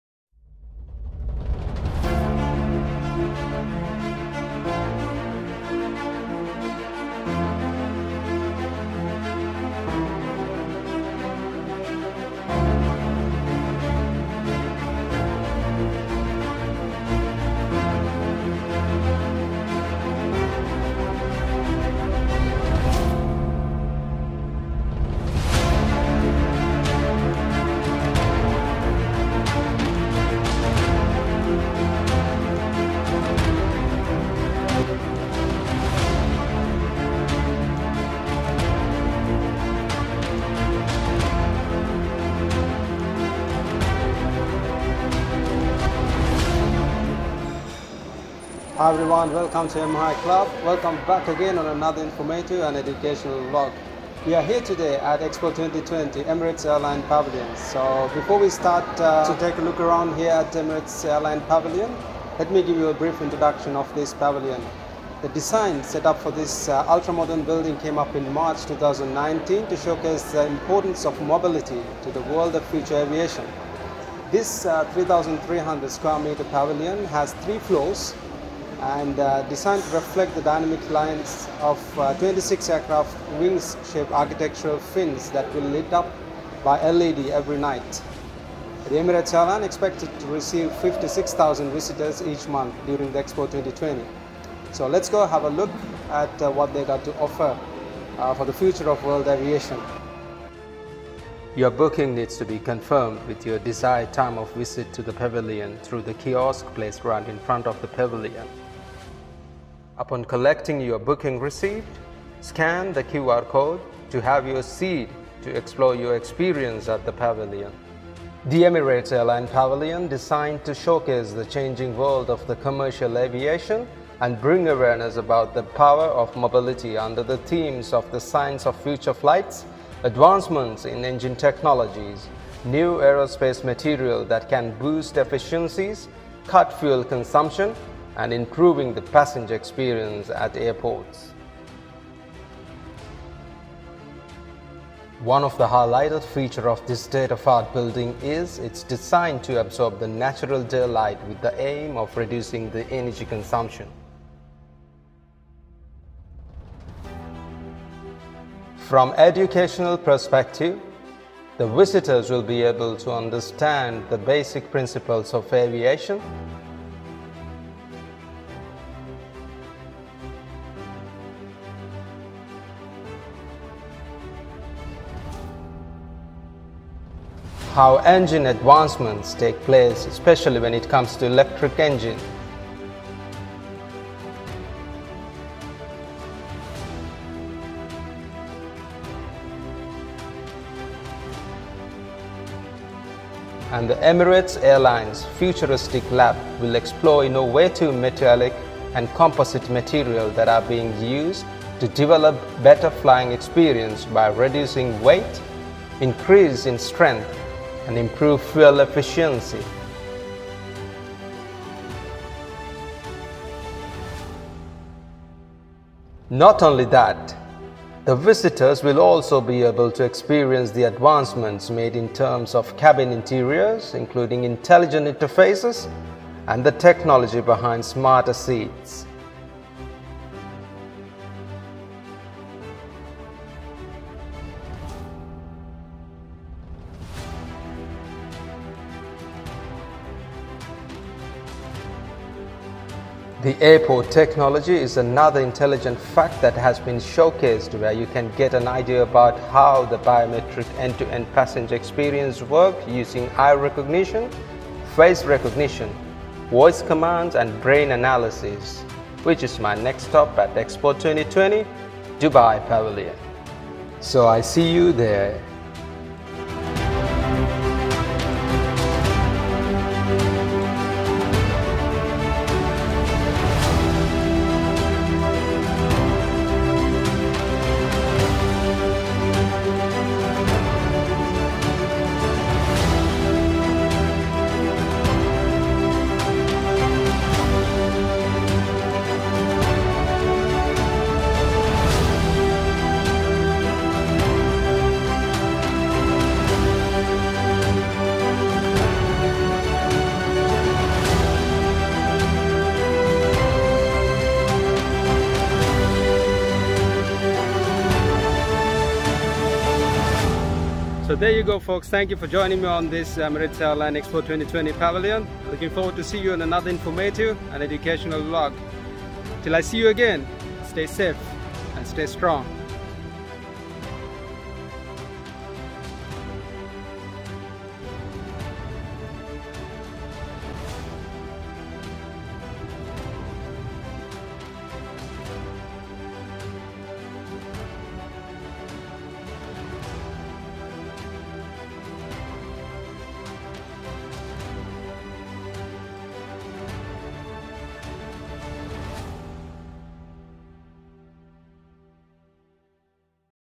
The Emirates Pavilion | EXPO 2020 Dubai
I had the privilege to visit Emirates Airlines Expo 2020 pavilion and all I can say it"s the next world of future aviation. I was thrilled by the theme of the pavilion "The Mobility", and how Emirates Airlines curated their experience throughout the journey.

From the advancements in engine technologies to signs of future flights and from basic aerodynamics to composite materials that are being used to develop better flying experience by reducing weight, increase strength and improve fuel efficiency was displayed ergonomically.

I recommend the Emirates Airlines Expo 2020 pavilion to all the aviation enthusiasts in the country and the region and for all the young and energetic academicians.